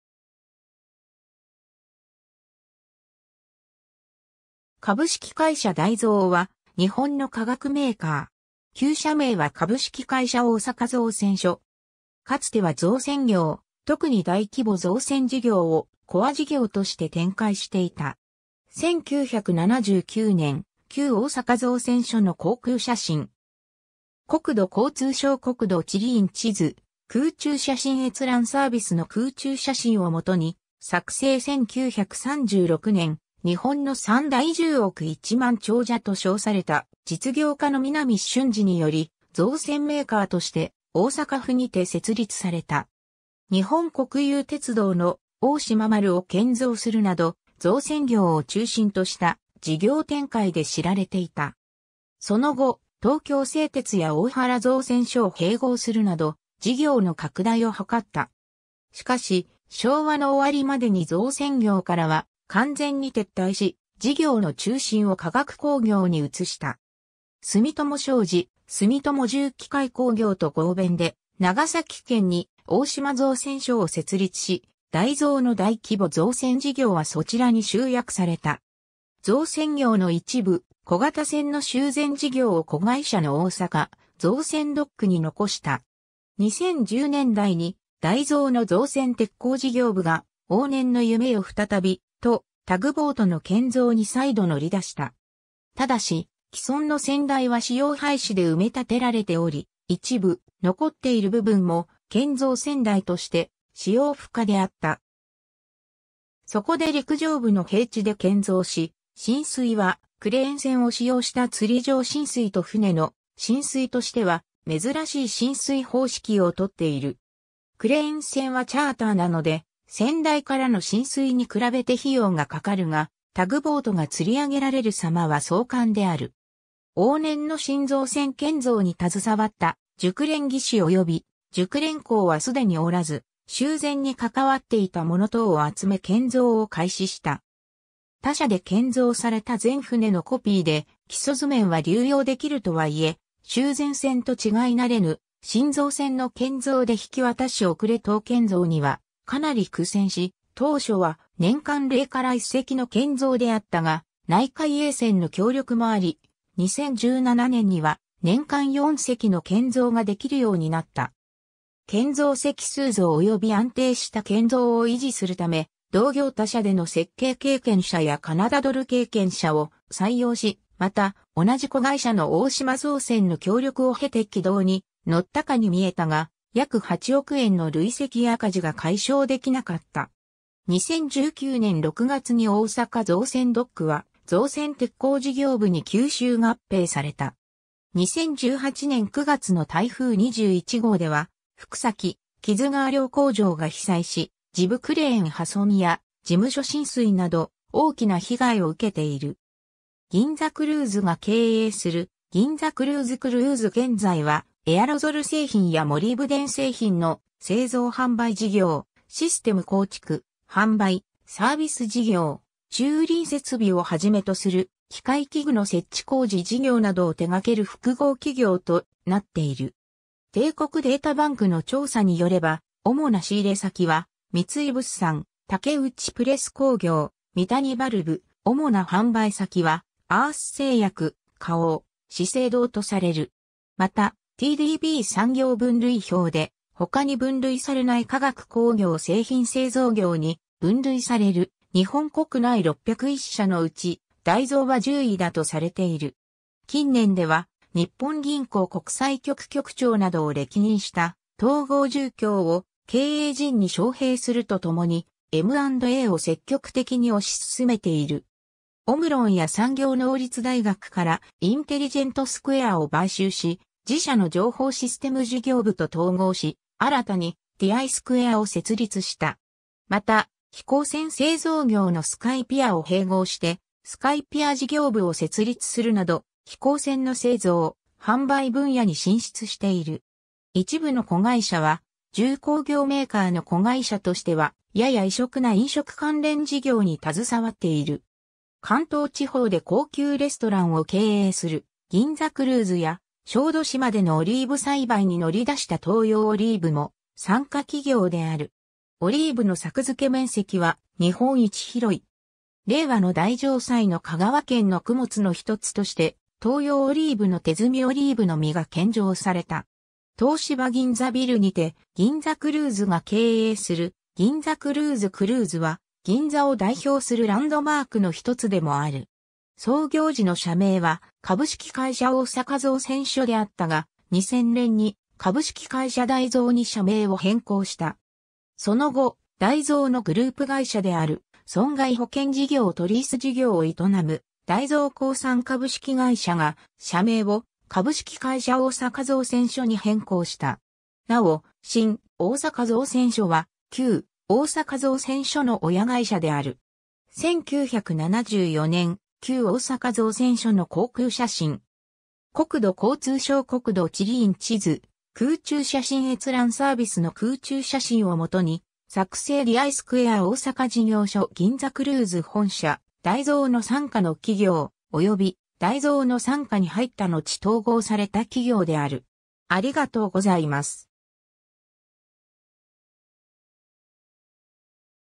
ダイゾー
株式会社ダイゾー（英語：Daizo Corporation）は、日本の化学メーカー。
 旧社名は株式会社大阪造船所（おおさかぞうせんじょ、英語：Osaka Shipbuilding Co.,Ltd.）。
かつては造船業、特に大規模造船事業をコア事業として展開していた。
 1979年、旧大阪造船所の航空写真。
国土交通省 国土地理院 地図・空中写真閲覧サービスの空中写真を基に作成 1936年、「日本の三大億万長者」と称された実業家の南俊二により、造船メーカーとして大阪府にて設立された。
日本国有鉄道の「大島丸」を建造するなど、造船業を中心とした事業展開で知られていた。
その後、東京製鉄や大原造船所を併合するなど、事業の拡大を図った。
 しかし、昭和の終わりまでに造船業からは完全に撤退し、事業の中心を化学工業に移した。
住友商事、住友重機械工業と合弁で長崎県に大島造船所を設立し、ダイゾーの大規模造船事業はそちらに集約された（船台の大部分は埋め立てられ、一部の工場とクレーンを除き解体され、跡地はコクヨ等に貸し出されている。時折残されたクレーンで阪急電車や新幹線の車体が陸揚げされていることから、鉄道ファンの聖地である）。
 造船業の一部、小型船の修繕事業を子会社の㈱大阪造船ドック（木津川）（1985年設立～2019年閉鎖）に残した。
 2010年代に、ダイゾーの造船鉄構事業部（実質建造は子会社の㈱大阪造船ドック（大阪府大阪市））が「往年の夢よ再び」、とタグボートの建造に再度乗り出した。
但し、既存の船台は使用廃止で埋め立てられており、一部残っている部分も建造船台として使用不可であった。
そこで陸上部の平地で建造し、進水はクレーン船を使用した吊上進水と船の進水としては珍しい進水方式をとっている。
クレーン船はチャーターなので船台からの進水に比べて費用が掛かるが、タグボートが吊り上げられる様は壮観である。
 往年の新造船建造にたずさわった熟練技師及び熟練工は既におらず、修繕に関わっていた者等を集め建造を開始した。
他社で建造された前船のコピーで基礎図面は流用できるとは言え、修繕船と違い慣れぬ新造船の建造で引渡し遅れ等建造にはかなり苦戦し、当初は年間0～1隻の建造であったが、内海曳船の協力もあり、2017年（平成29年）には年間4隻の建造が出来る様になった。
建造隻数増及び安定した建造を維持するため、同業他社での設計経験者やCAD経験者を採用し、また、同じ子会社の大島造船の協力を経て軌道に乗ったかにみえたが、約８億円の累積赤字が解消できなかった。
2019年06月に㈱大阪造船ドックは造船鉄構事業部に吸収合併された。
 2018年9月の台風21号では、福崎、木津川両工場が被災し、ジブクレーン破損や事務所浸水など大きな被害を受けている。
 銀座クルーズが経営する「GINZA CRUISE CRUISE」 現在はエアロゾル製品（日本のエアゾール製品の3本に1本を製造。日本市場の専有率は一番高い。）やモリブデン製品の製造販売事業（潤滑用MoS2粉の販売量は日本市場の70%。パーツクリーナの出荷数量は日本市場で１位）、システム構築・販売・サービス事業、駐輪設備をはじめとする機械器具の設置工事事業などを手がける複合企業となっている。
帝国データバンクの調査によれば、主な仕入先は三井物産、武内プレス工業、三谷バルブ、主な販売先はアース製薬、花王、資生堂とされる。
また、TDB産業分類表で「他に分類されない化学工業製品製造業」に分類される日本国内601社のうち、ダイゾーは10位だとされている。
 近年では、日本銀行国際局局長などを歴任した東郷重興を経営陣に招聘するとともに、M&Aを積極的に推し進めている。
オムロンや産業能率大学からインテリジェントスクエアを買収し、自社の情報システム事業部と統合し、新たにディアイスクエアを設立した。
また、飛行船製造業のスカイピアを併合してスカイピア事業部を設立するなど、飛行船の製造・販売分野に進出している。
 一部の子会社は、重工業メーカーの子会社としてはやや異色な飲食関連事業に携わっている。
関東地方で高級レストランを経営する銀座クルーズや、小豆島でのオリーブ栽培に乗り出した東洋オリーブも傘下企業である。
オリーブの作付け面積は日本一広い。
令和の大嘗祭の香川県の供物の一つとして東洋オリーブの手摘みオリーブの実が献上された。
東芝銀座ビルにて銀座クルーズが経営する「GINZA CRUISE CRUISE」は、...